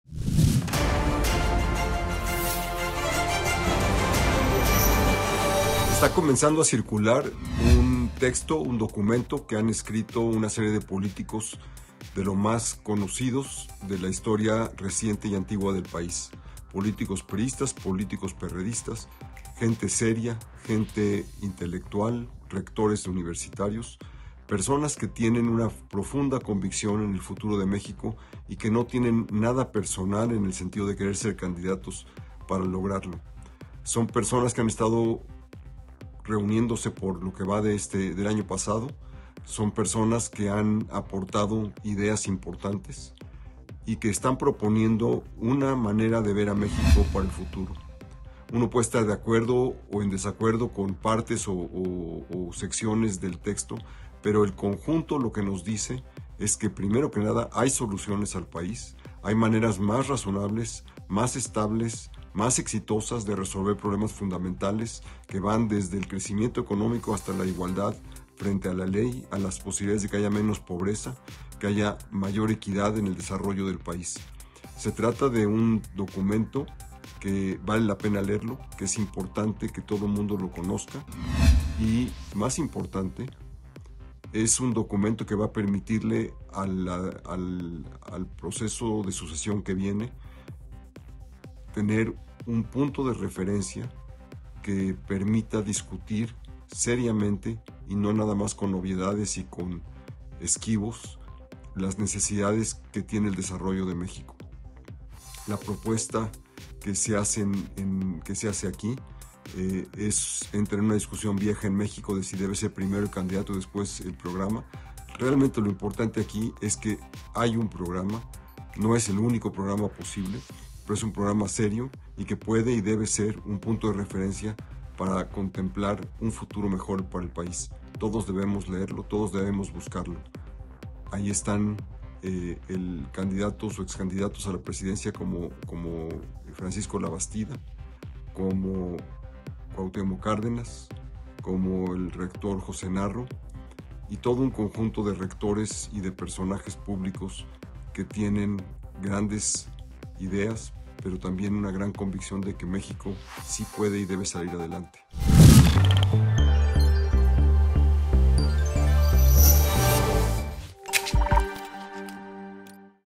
“Colectivo por México”: políticos, excandidatos y rectores se unen a favor de México
Luis Rubio reflexiona sobre el "Colectivo por México" escrito por una serie de políticos conocidos y explica por qué hay que leerlo.